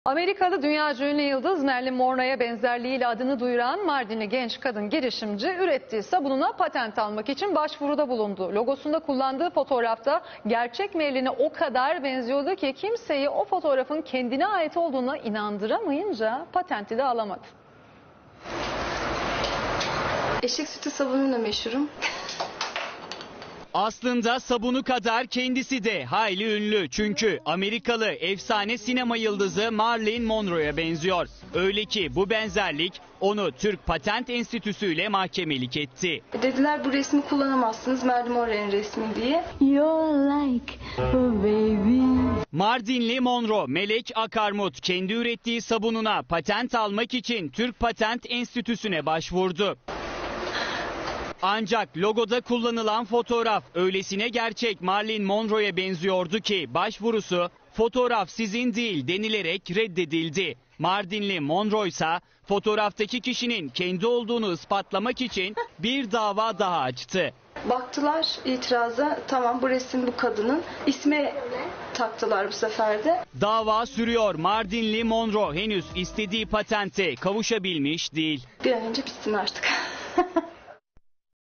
Benzerliği yüzünden mahkemelik oldu
AMERİKALI DÜNYACA ÜNLÜ YILDIZ MARLİYN MONROE'YE BENZERLİĞİ İLE ADINI DUYURAN MARDİNLİ GENÇ KADIN GİRİŞİMCİ,,, ÜRETTİĞİ SABUNUNA PATENT ALMAK İÇİN BAŞVURUDA BULUNDU. LOGOSUNDA KULLANDIĞI FOTOĞRAFTA ÖYLESİNE ÇOK GERÇEK MARILYN'E BENZİYORDU Kİ KİMSEYİ O FOTOĞRAFIN KENDİNE AİT OLDUĞUNA İNANDIRAMAYINCA PATENTİ ALAMADI.